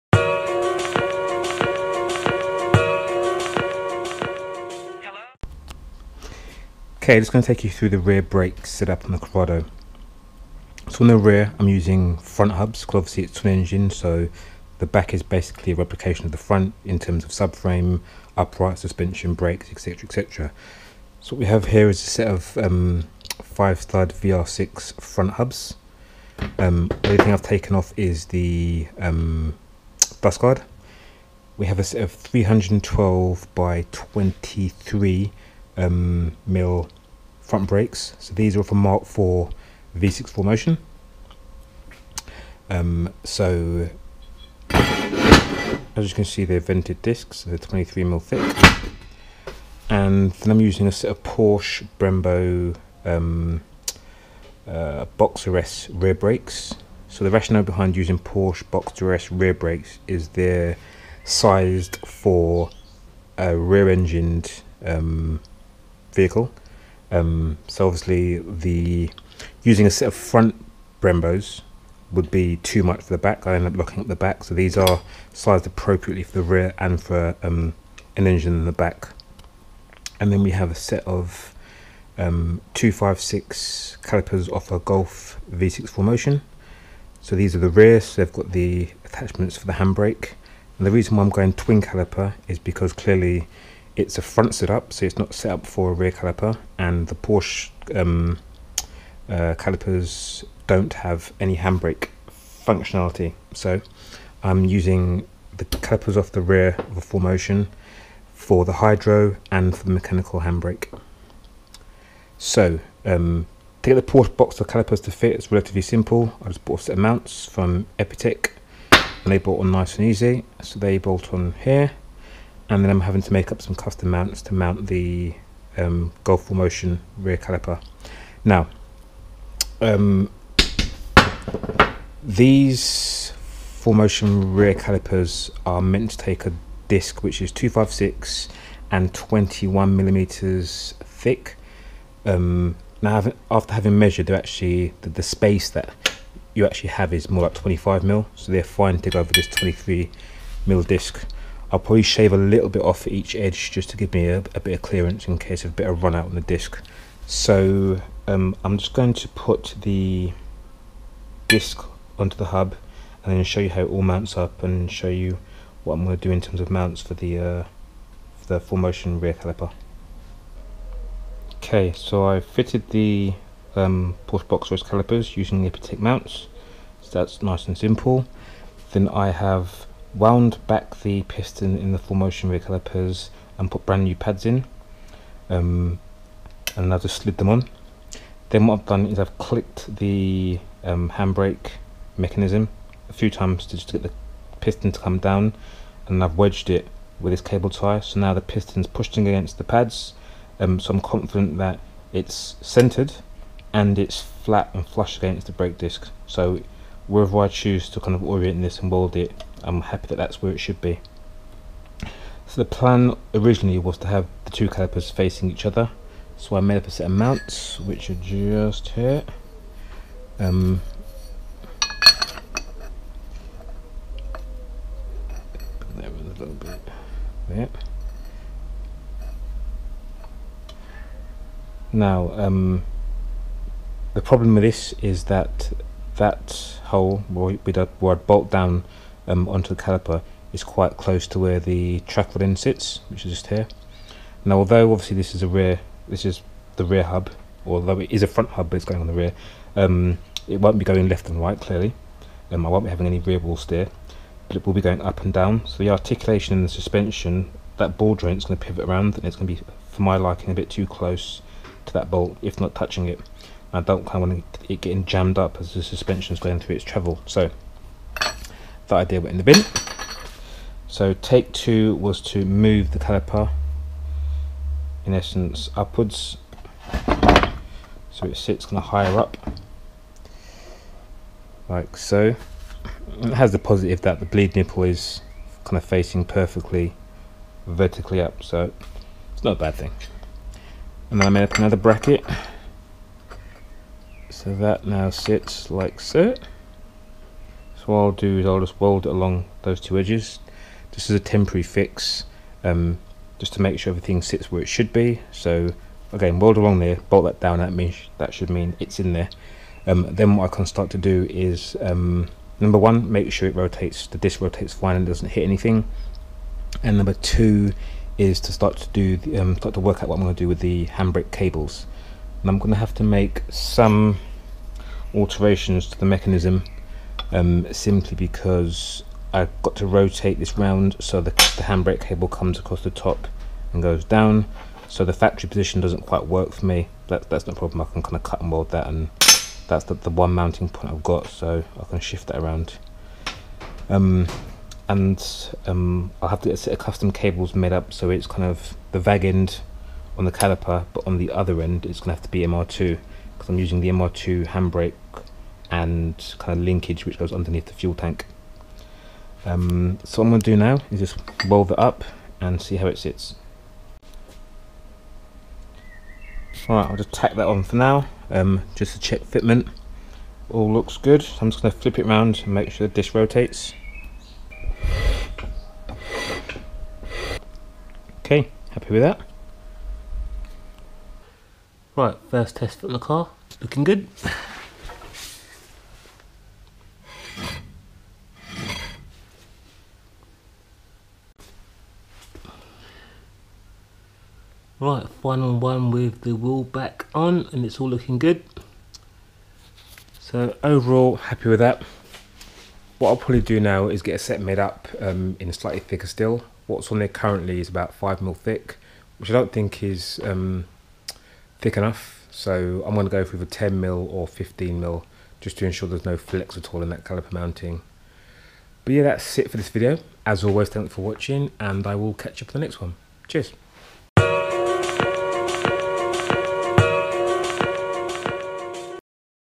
Dual Rear Caliper Set Up - Twin Engined Corrado VR6
Made some brackets to mount the dual calipers to the rear uprights